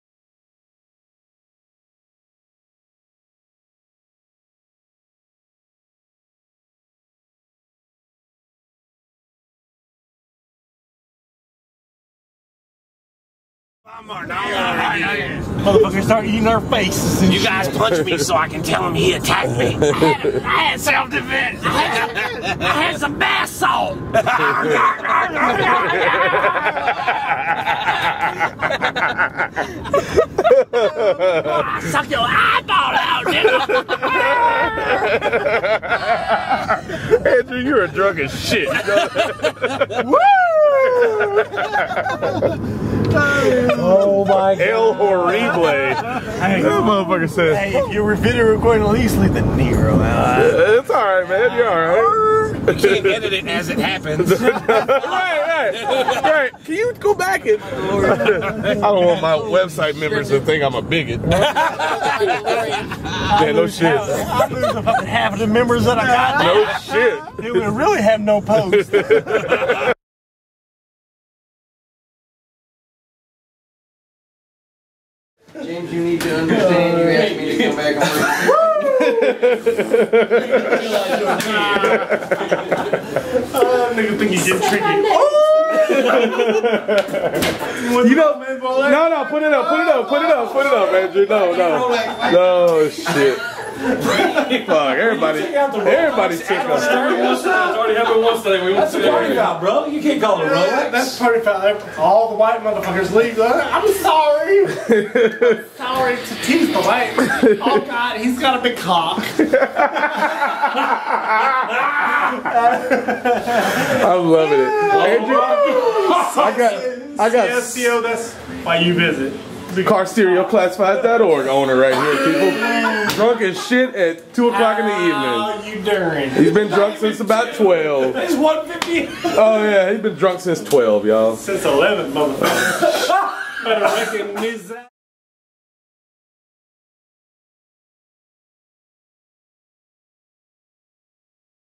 Belligerent DRUNK! Andrew....Owner of Carstereoclassifieds.org! Demo King Woes
Here is my good buddy Andrew. great guy. Only problem is .he HATES the fact that the people crowned me Demo KIng :( To num the pain he drinks and drinks and drinks. I have documented these occasions. it's not pretty. Be sure to go check out his forum for all your car audio needs! With a owner like this. how could you resist?